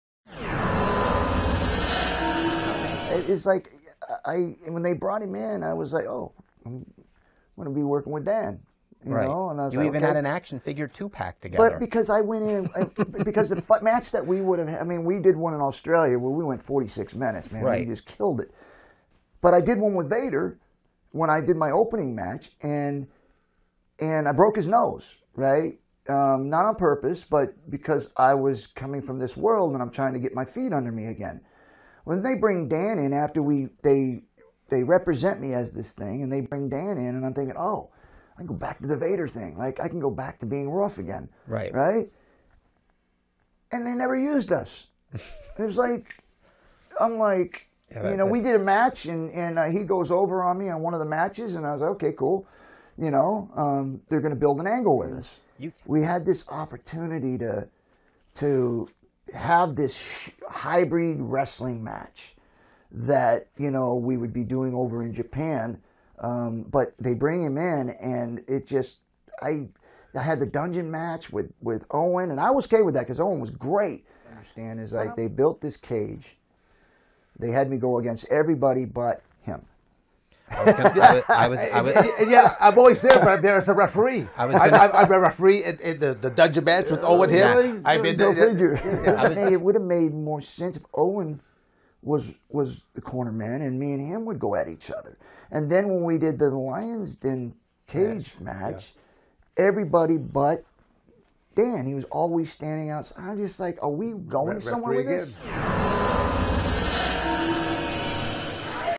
Ken Shamrock & Dan Severn - Why WWF Never Used Us in a Program Together
Ever wonder why WWF never did anything with Ken Shamrock & Dan Severn when they were both fresh off of UFC? Stream the Full Shoot Interview 📺

Former WWF/WWE/UFC legends Ken Shamrock & Dan Severn are here in their very first shoot interview together.

Severn and Shamrock fought each other in the octogen back when UFC was in its infancy. These two were the first real crossover talents who bridged the gap between pro wrestling and mixed martial arts.

Both Ken Shamrock & Dan Severn figured that once Vince McMahon signed them to WWF during the Attitude Era, a program between the two would be a no-brainer.

However, nothing ever transpired and the two biggest crossover attractions from UFC would not wrestle a single program together.

Originally produced by RF Video Inc. Licensed for distribution on Title Match Network.